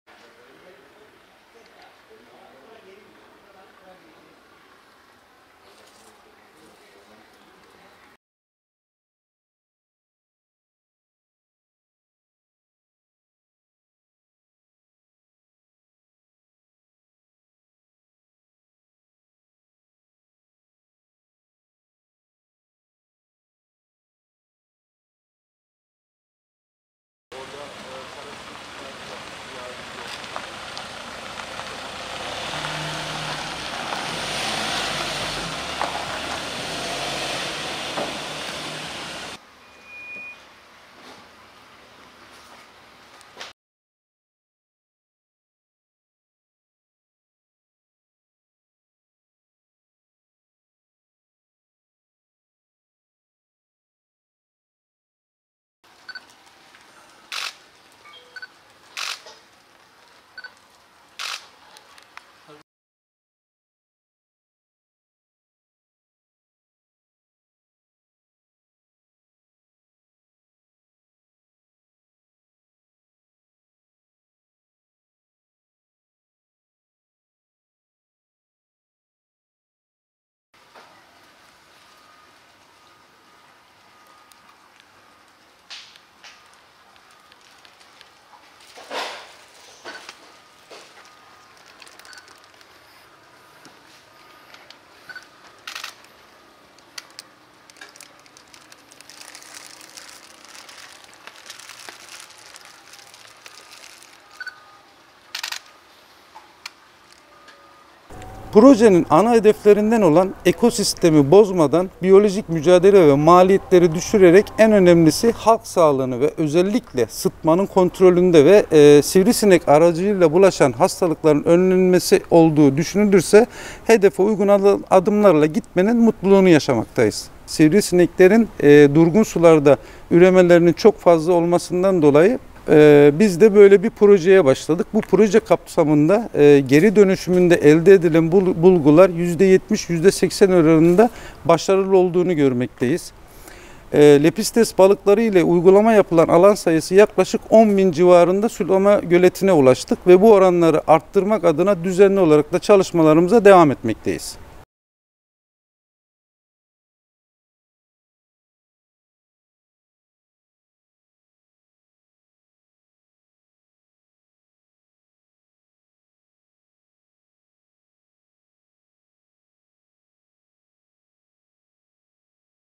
BÜYÜKŞEHİR’DEN SİVRİSİNEKLE MÜCADELEDE ETKİLİ YÖNTEM
Mersin Büyükşehir Belediyesi, 2020 yılında sivrisinekle mücadele için başlattığı lepistes balığı üretiminde, önemli aşama kaydetti. Bin havuzla başlayan proje genişleyerek, 10 bin havuza ulaştı. Havuzlara bırakılan binlerce lepistes balığı larvaları yiyerek sivrisineklerin üremesinin önüne geçerken, biyosidal ürün kullanımı da sıfıra indiriliyor.